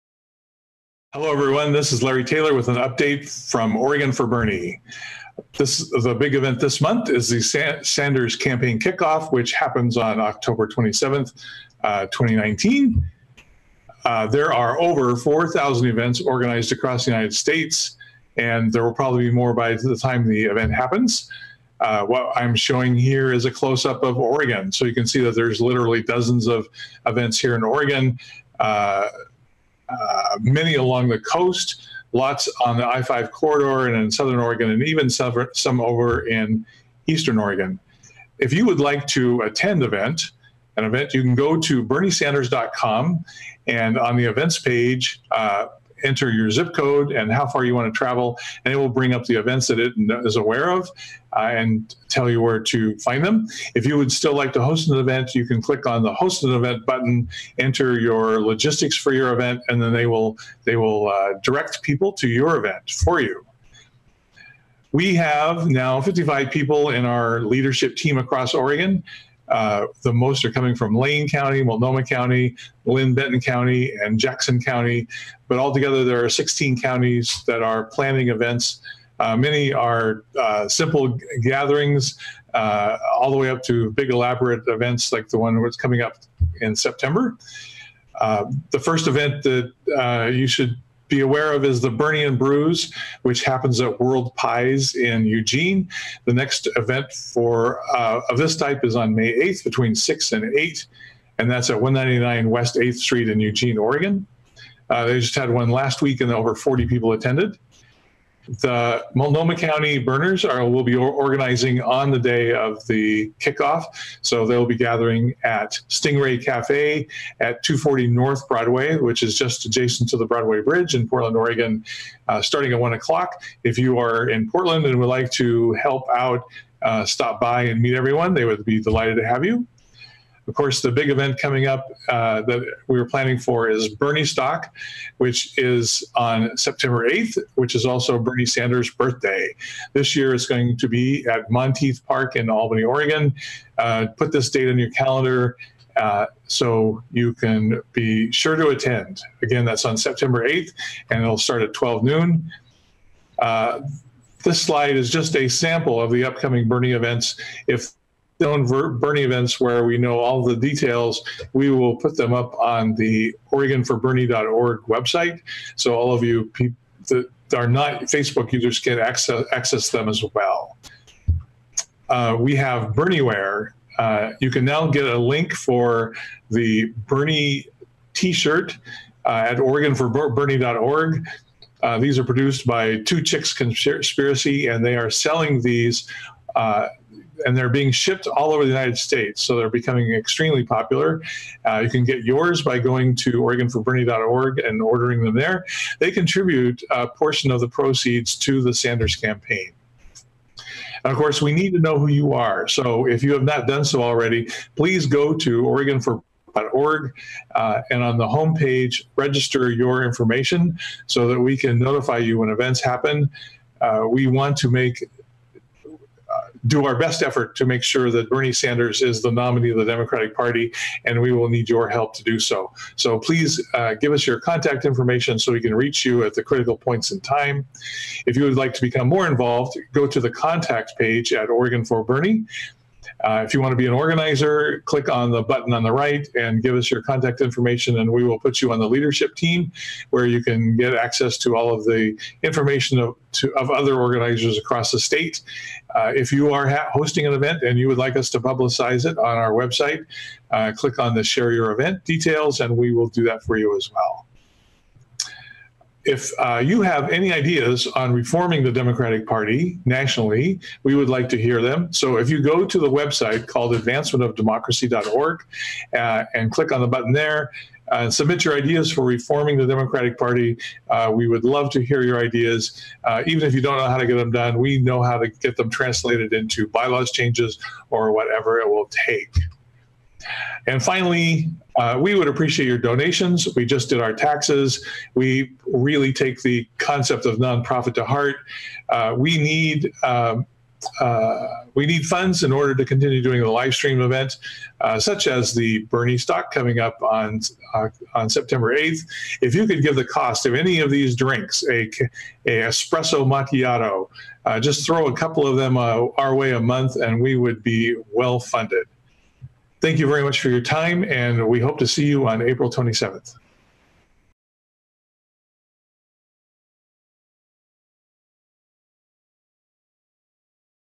Oregon for Bernie Update April 14th, 2019
Larry Taylor gives a quick update about what's going on in Oregon to support Bernie Sanders 2020 campaign.